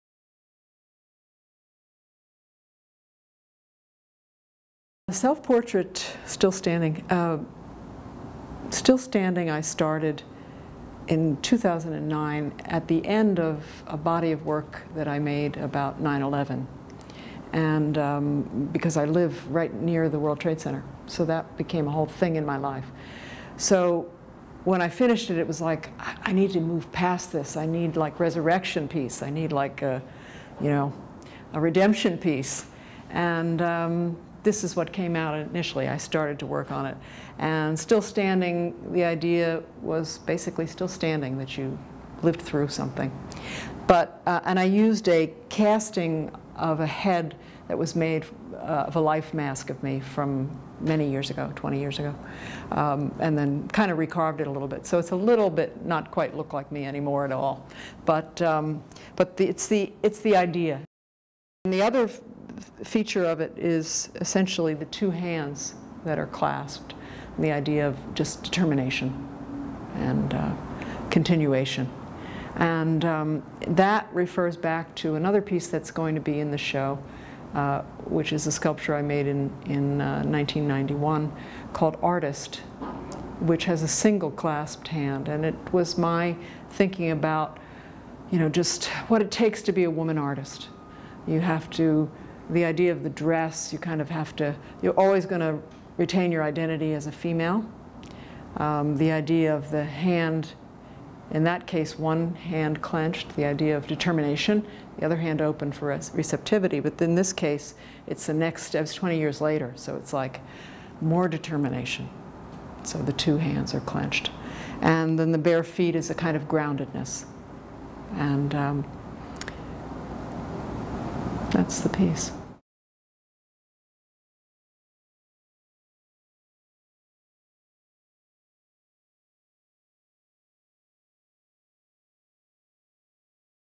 Judith Shea NA on her self portrait, Still Standing shown in Her Own Style at the National Academy
Drawn from the National Academy of Design's extensive portrait collection, Judith Shea NA selected a group of women artists' portraits spanning from the late 19th century to today for Her Own Style: An Artist's Eye with Judith Shea.  Shea created two original sculptures for the exhibition, one of Elizabeth Catlett and one of Louise Bourgeois, two similarly iconoclastic and groundbreaking artists.

Video by David Plakke Media